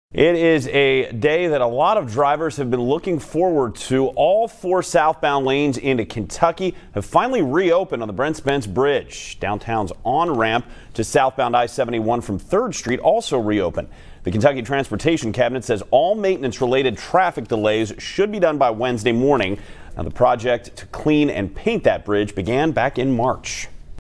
Southbound lanes over Brent Spence Bridge fully reopen
It is a day that a lot of drivers have been looking forward